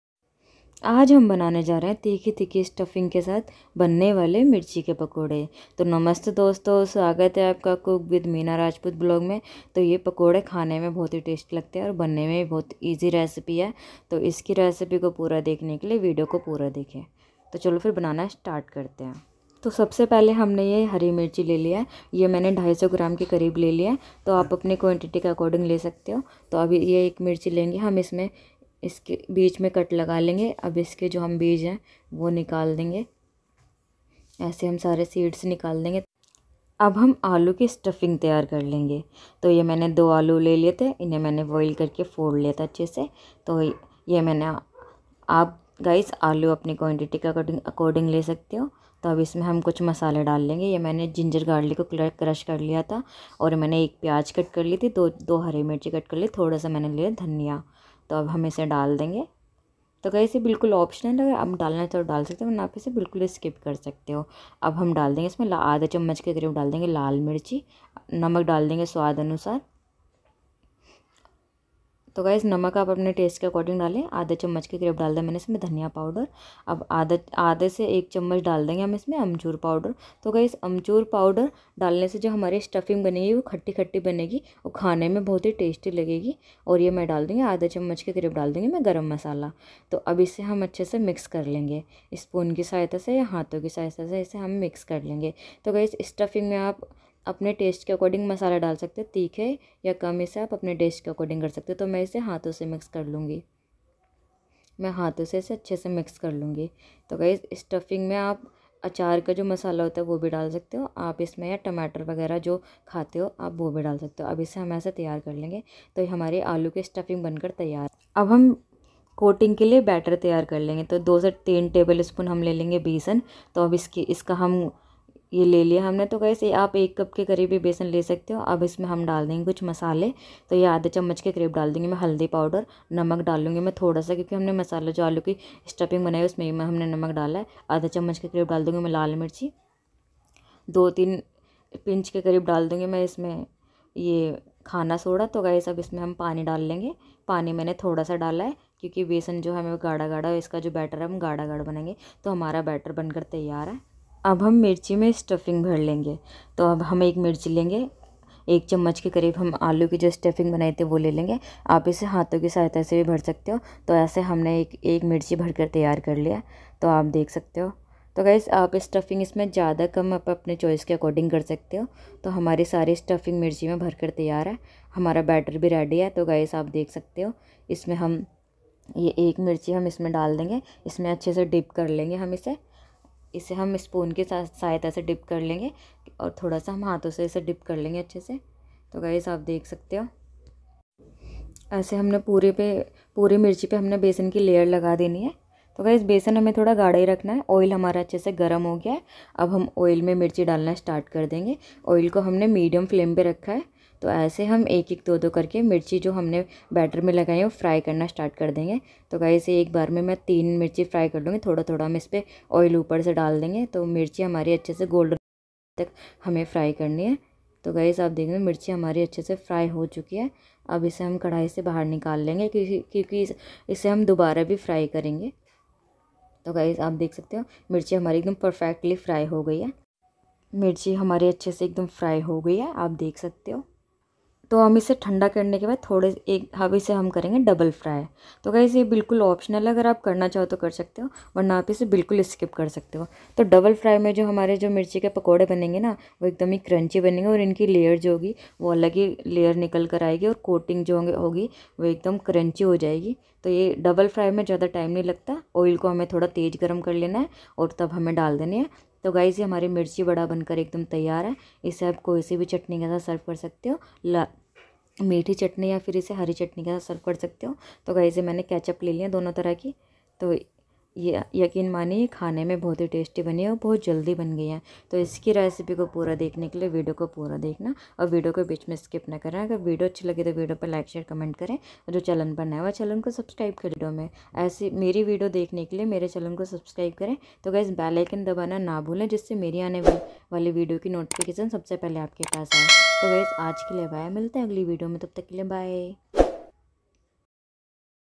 राजस्थान के फेमस मिर्ची बड़ा रेसिपी mirch ke pakore
Please check my other recipe:-

Ingredients.
Thick green chillies:- 250gm
Onion:- 1
Aamchur powder 1tsp
Salt to taste
Baking soda:- 1 to 2pinc
Cooking oil to fry
Coriander leave